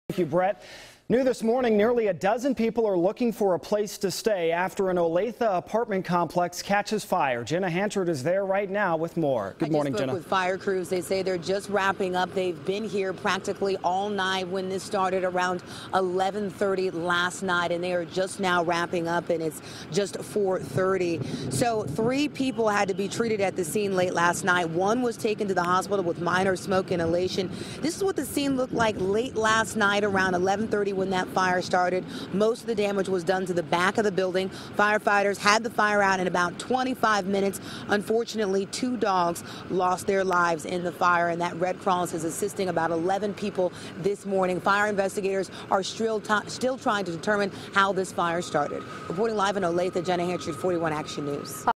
11 displaced bu JoCo fire; 2 dogs killed
Four families were displaced after an apartment fire in Olathe Thursday night. Two dogs were killed in the blaze.